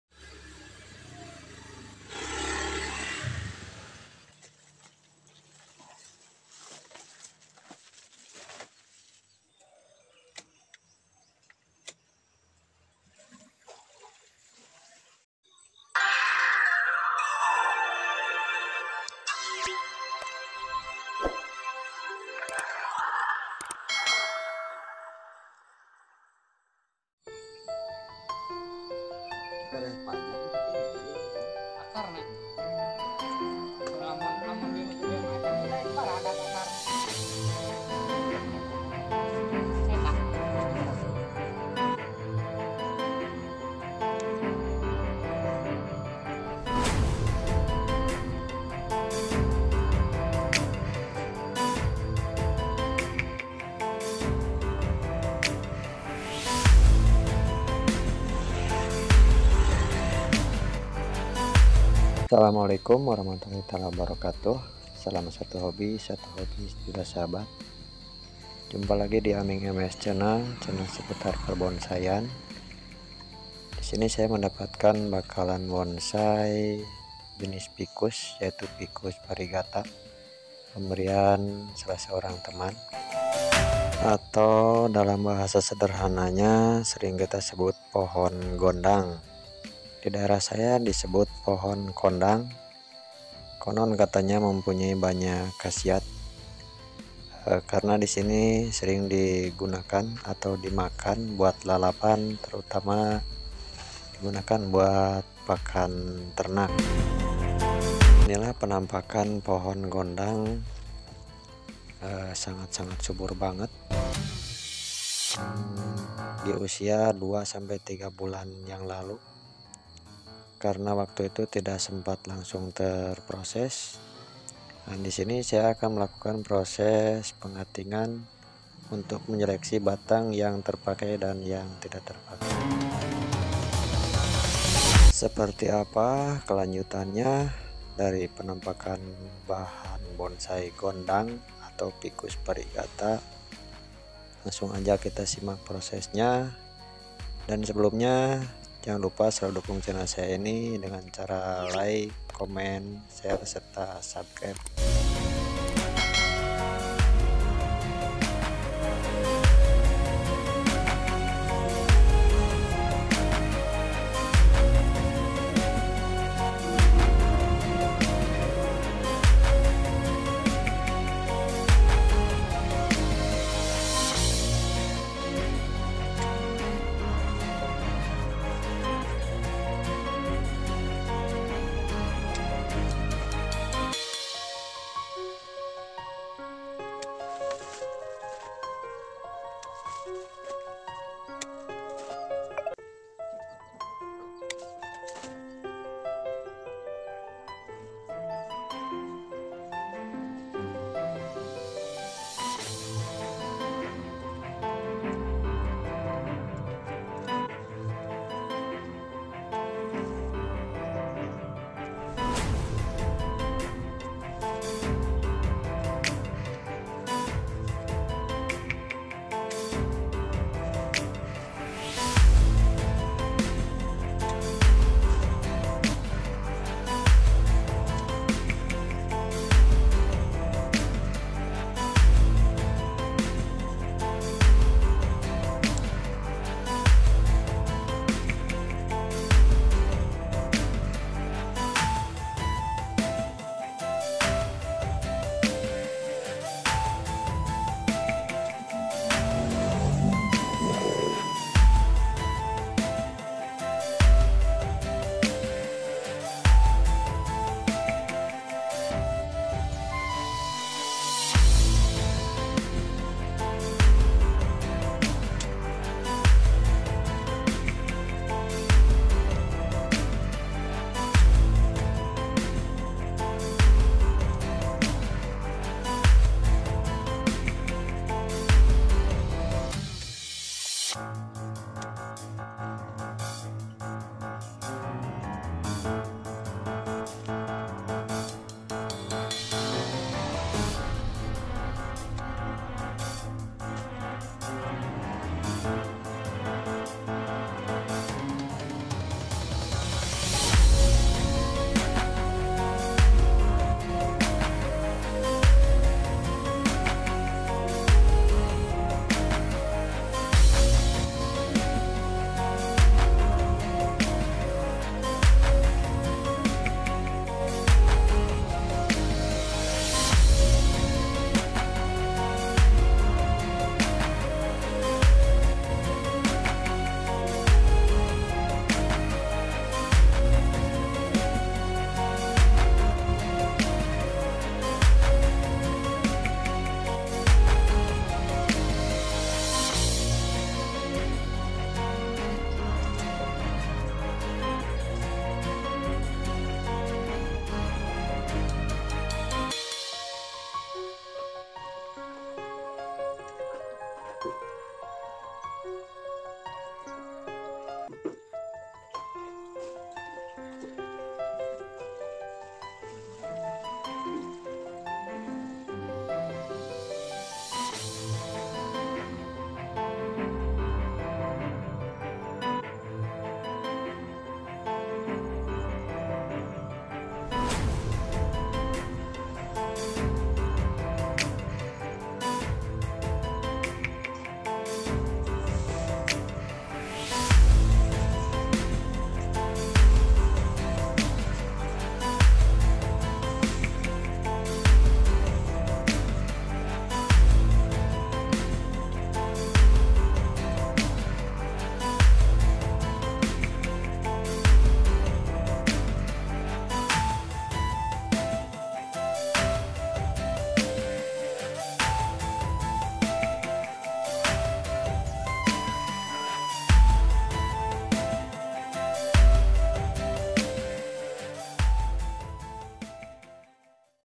Tahap lanjutan, ba~bon || GONDANG/kondang (ficus variegata) || hasil adopsi dari teman
tahap lanjutan bakalan bonsai gondang atau kondang,(ficus variegata) hasil pemberian dari teman, coba coba di bikin bonsai, karna pohon gondang di daerah saya jarang di bikin bonsai, dan juga udah mulai langka,